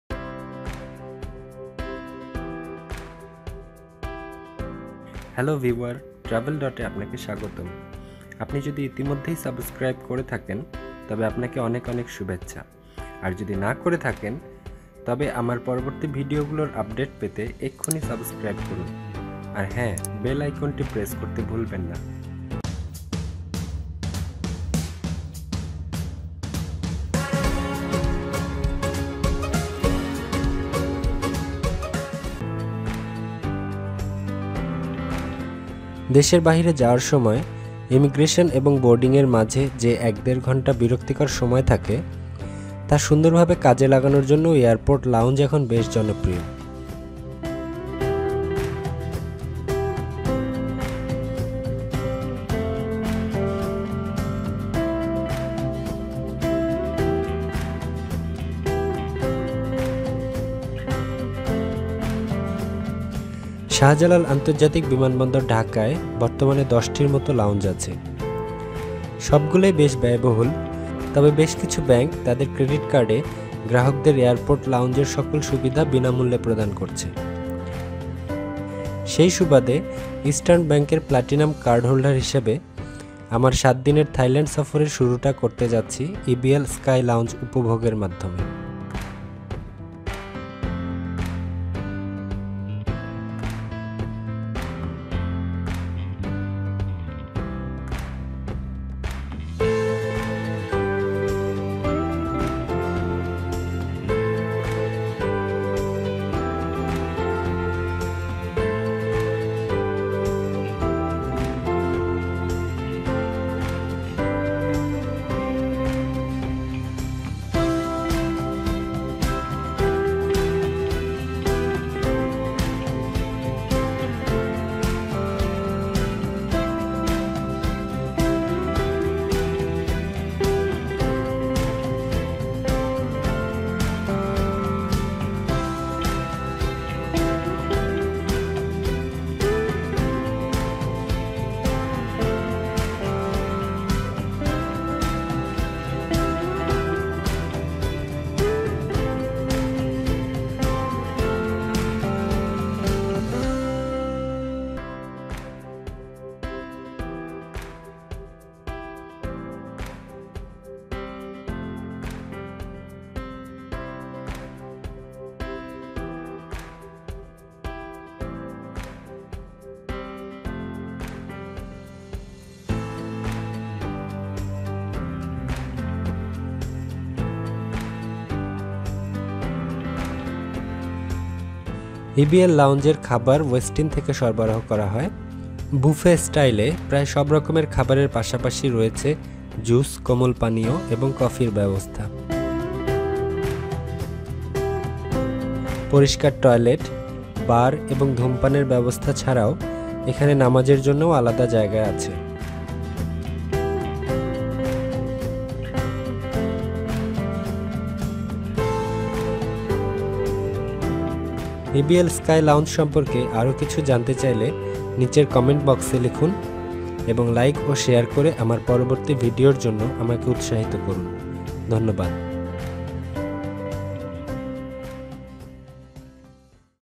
EBL lounge dhaka airport | Bangla | থাইল্যান্ড ২০১৯ - পর্ব ১
Airport lounges have become very popular among tourists to enjoy the boring time between immigration and boarding. Hazrat Shahjalal International Airport Dhaka currently houses 10 lounges. All of them are very much expensive. But many banks are offering access and enjoyment of all facilities of airport lounges for free of cost. As a platinum card holder of Eastern Bank Limited, I am starting my 7 days trip to Thailand by enjoying EBL SkyLounge at Dhaka International airport.

EBL lounge offers a range of food in buffet system. Also, a range of juice, soft drinks and coffee is available.

Aside from clean toilets, bar, smoking area, it also offers a place for prayers.

If you want to know more about EBL lounge, please comment in the below section.

Thanks